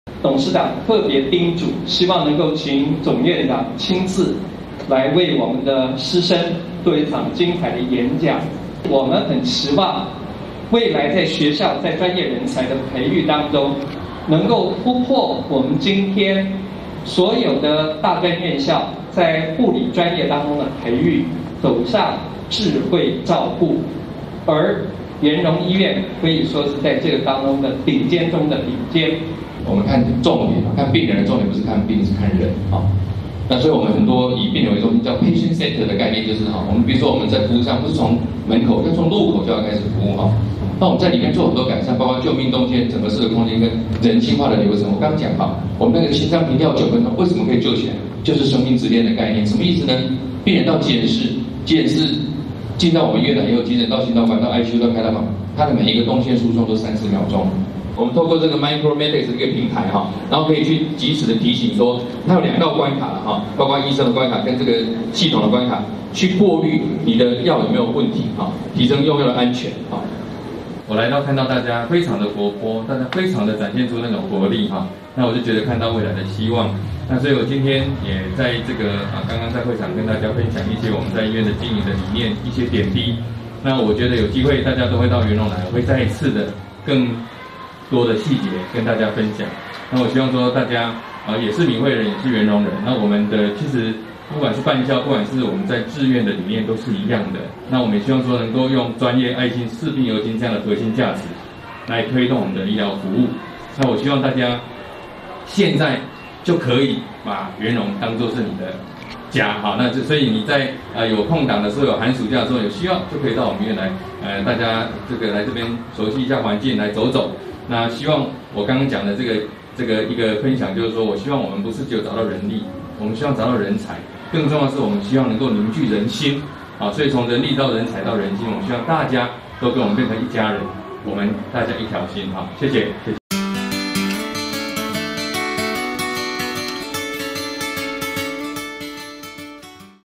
員榮醫療體系與敏惠醫專產學合作 培育智慧醫療人才
員榮醫療體系與敏惠醫專產學合作 培育智慧醫療人才 〔記者林明佑／員林市報導〕員榮醫療體系為延攬優秀醫護專業人才，與台南敏惠醫專合作，建置「智慧醫療與照護人才」培育，在醫療與健康科技進行跨領域合作，共同組成學術研究、產學合作專業團隊，並為員榮醫療體系增添新血輪。敏惠醫專護理科專四同學即將進入醫療院所為期一年的專業實習前夕，員榮醫療體系總院長張克士特別蒞校以「智慧醫療在醫院的實踐與發展」為題，勉勵護理師生。@changhuanews